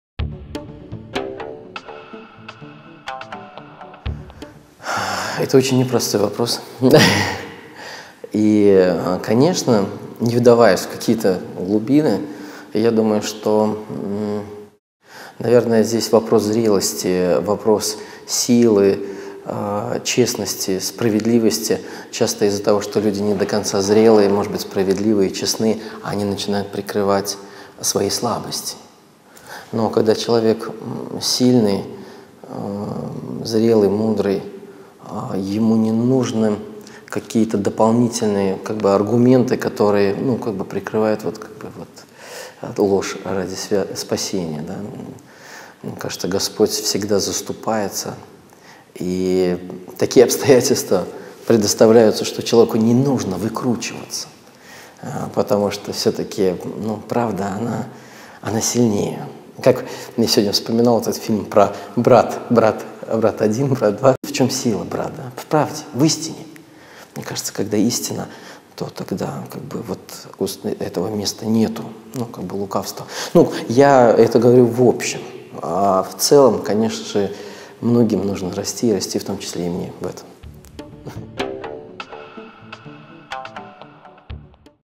Вопрос - ответ Вячеслав Глухов
Вячеслав Глухов отвечает на вопрос:"Допустима ли "ложь во спасение"?"